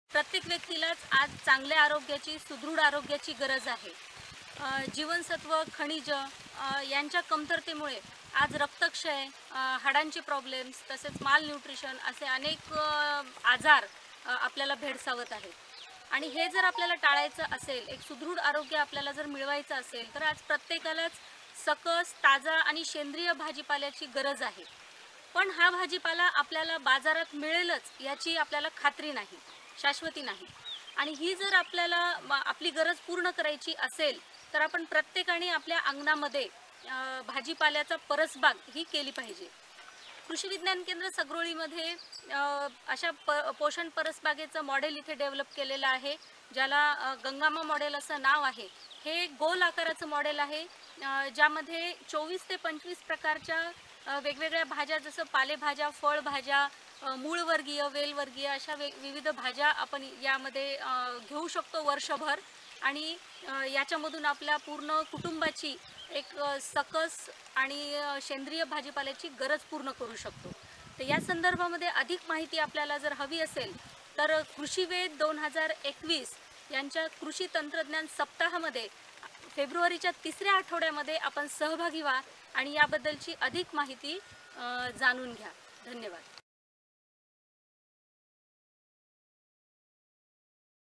कृषीवेद-2021 कमीतकमी जागेत अनेक प्रकारचा भाजीपाला लागवड करून वर्षभर आपली गरज भागविण्यासाठी पोषण बाग.
विशेष आकर्षण - पोषण बाग, दैनंदिन आहारात दर्जेदार व सकस असा सेंद्रिय भाजीपाला उपलब्ध होणे प्रत्येक कुटुंबाची गरज झाली आहे,आपल्या अंगणातच कमीतकमी जागेत अनेक प्रकारचा भाजीपाला लागवड करून आपली गरज भागविण्यासाठी गंगाम्मा मंडल पोषण बाग हे ऊत्तम उदाहरण आहे या बद्दल अधिक माहिती जाणून घेण्यासाठी प्रत्येक्ष भेट द्या फेब्रुवारी च्या 3 ऱ्या आठवड्यात कृषि वेद 2021,या  कृषी तंत्रज्ञान  सप्ताह कार्यक्रमास.